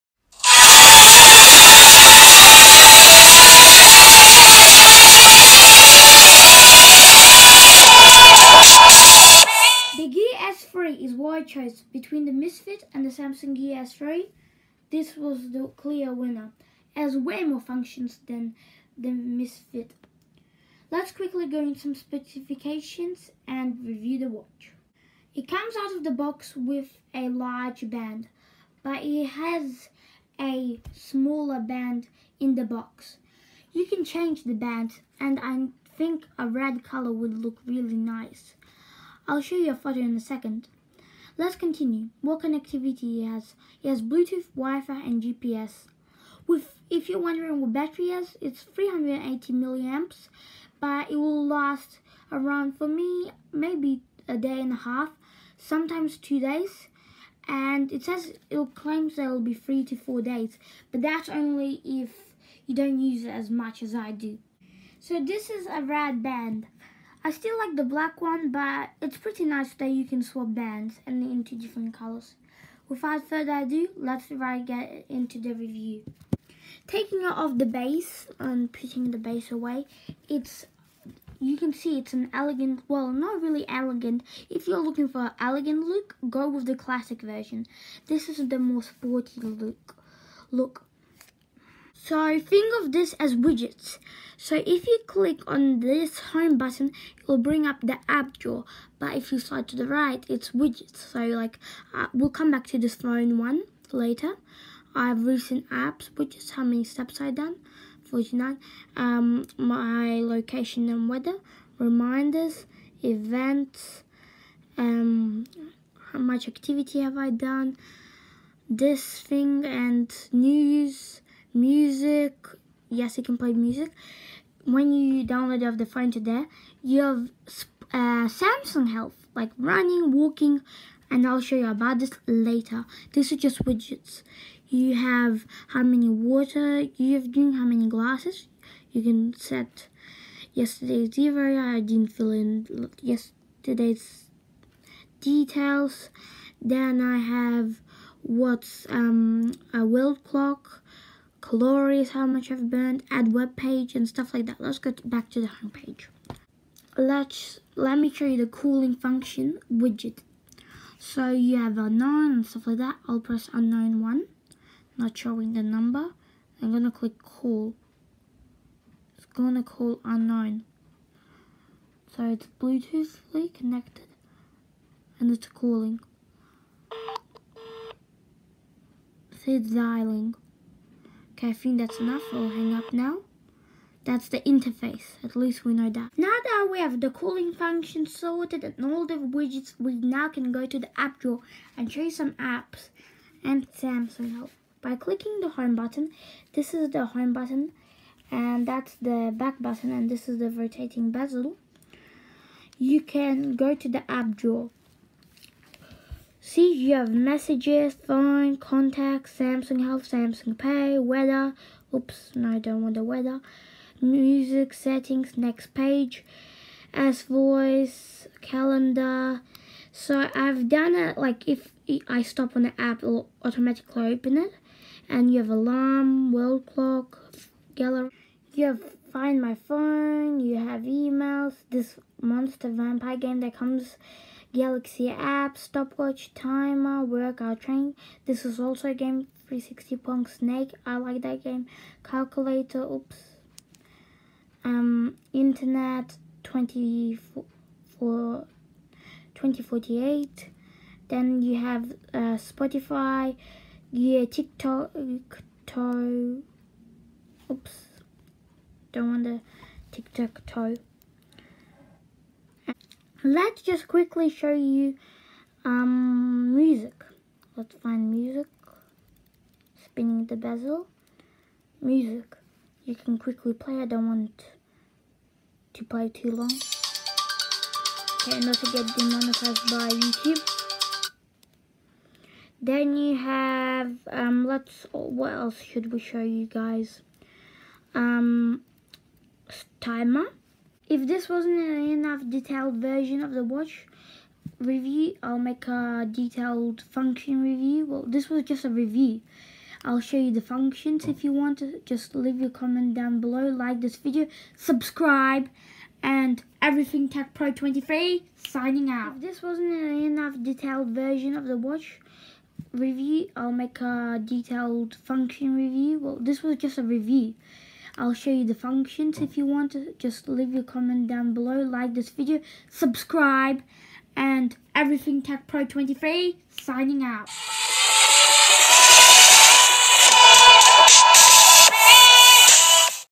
Samsung Gear S3 Review
Watch my opinion on this smart smart watch as it stands out from the other smart watches. It is a value for your money smartwatch. And great performance